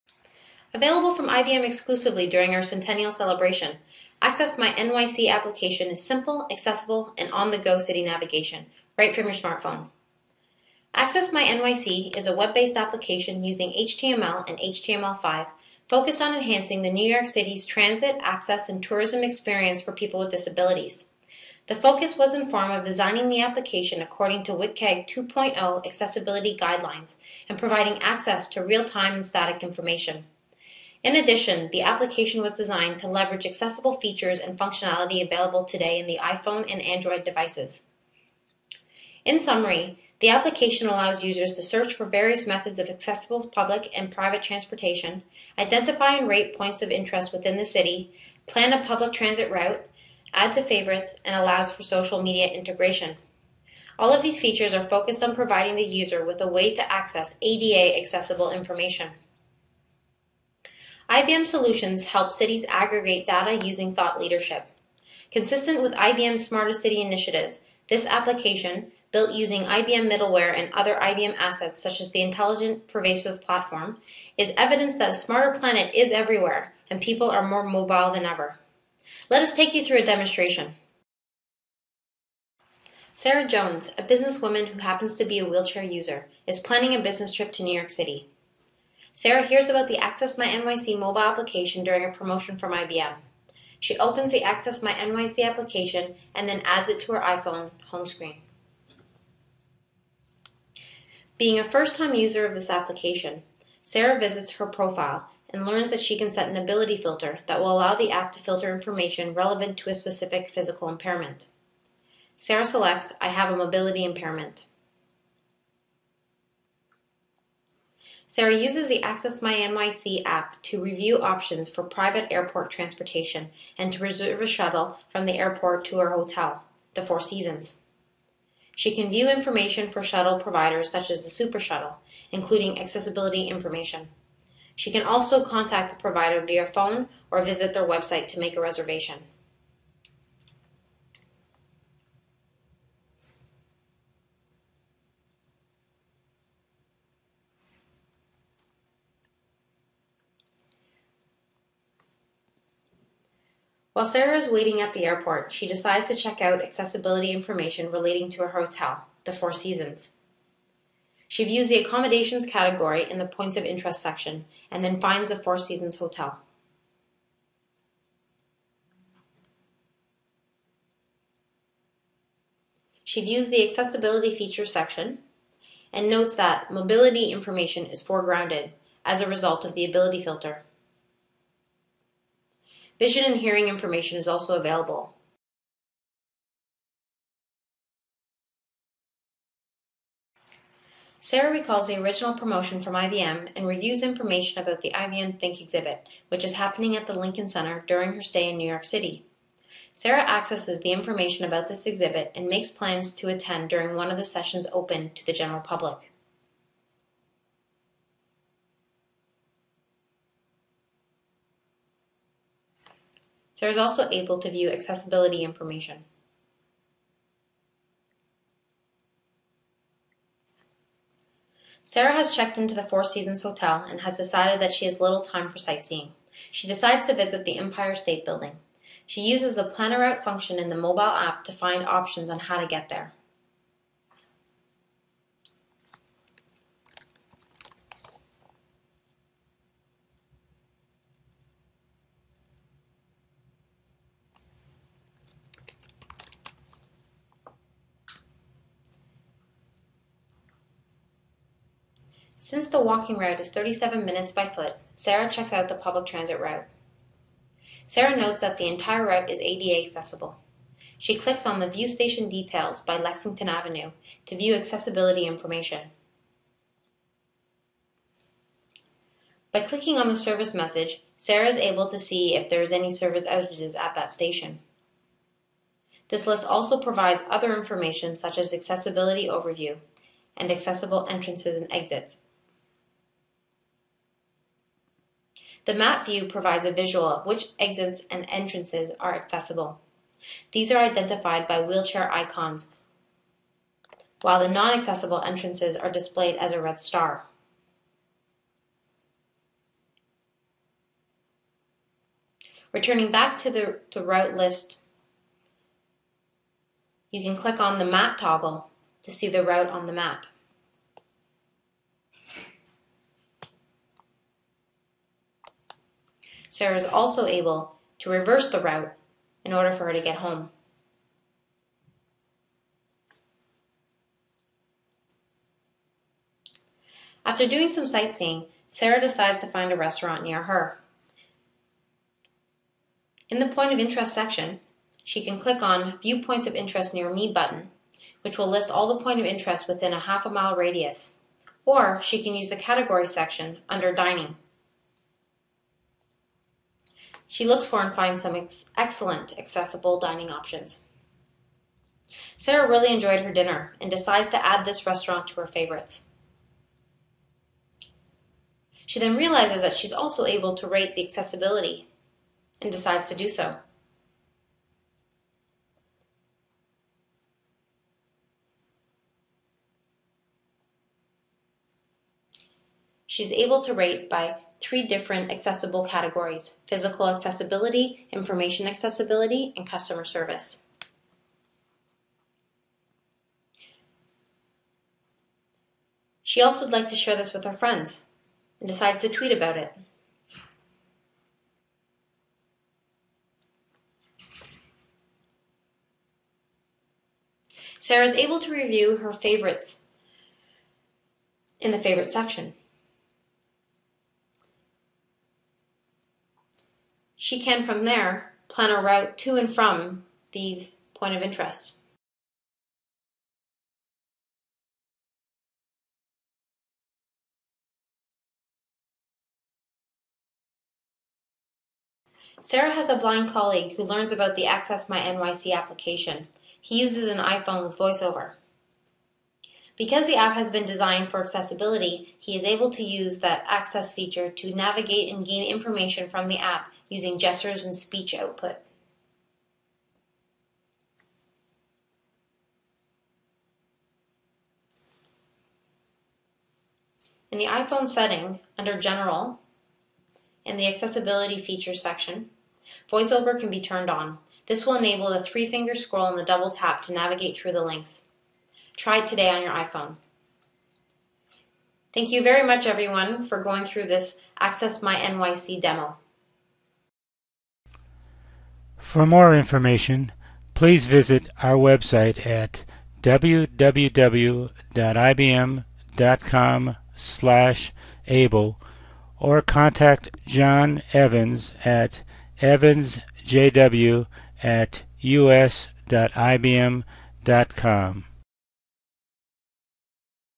IBM Access My N.Y.C. accessible pilot mobile travel application
Designed to help IBM and municipal leaders better define requirements for more inclusive Smarter Cities, Access My N.Y.C. brings together geo-location and mapping technologies, transportation data, and publicly available accessibility information to help N.Y.C. residents and visitors:
* Find information about accessible public and private transportation and plan a public transit or walking route
* Identify accessibility information about points of interest citywide, including restaurants, hotels and most commonly visited attraction
* Customize search criteria to filter accessibility information by personal needs and preferences
 * Rate and tweet about the accessibility of city systems and points of interest.